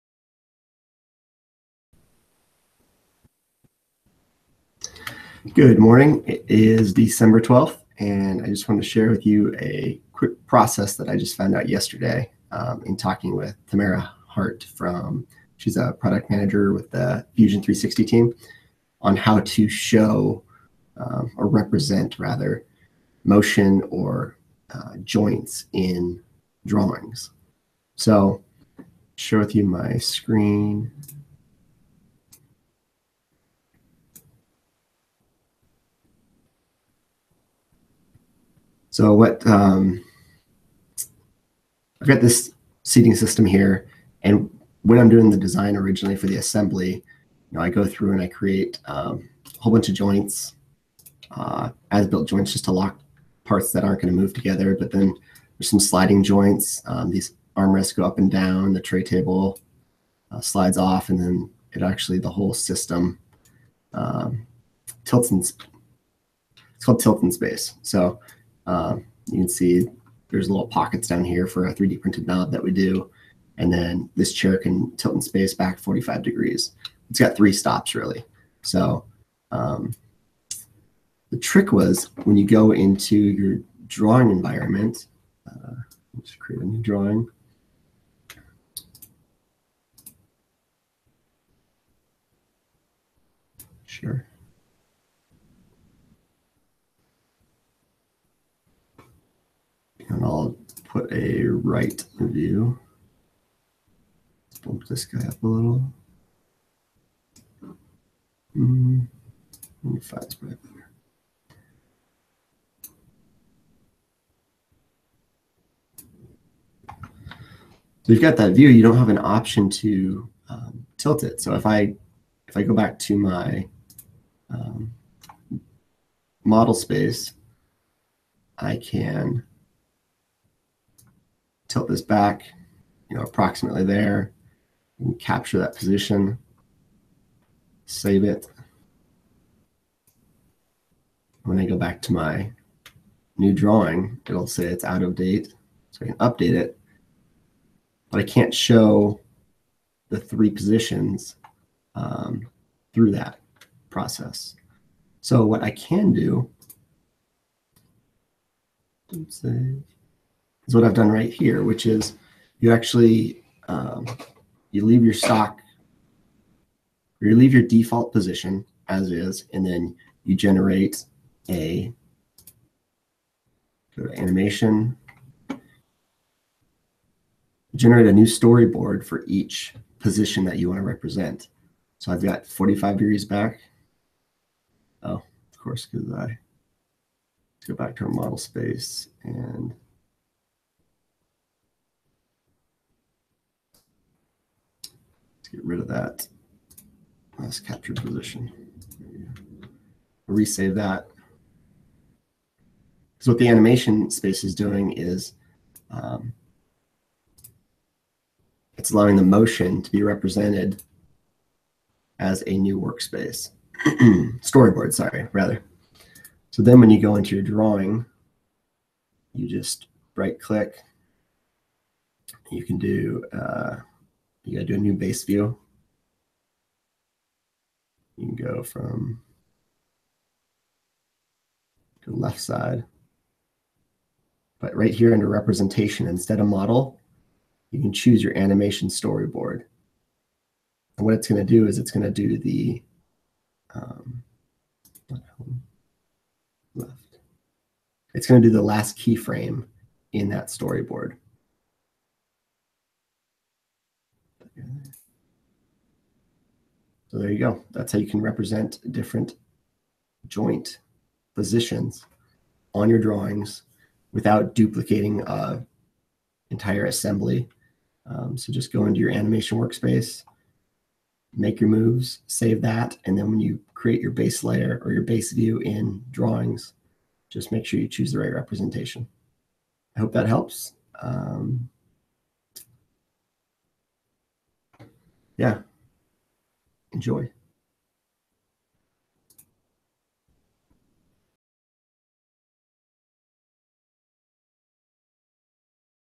Fusion 360 Drawings - Show Joint Range
When you create an assembly in Fusion 360 that utilizes joints, it can be difficult to demonstrate the motion to others on the team if you can't put them in front of the model. This process enables you to represent the motion in a drawing that you can print or email to the members of your team.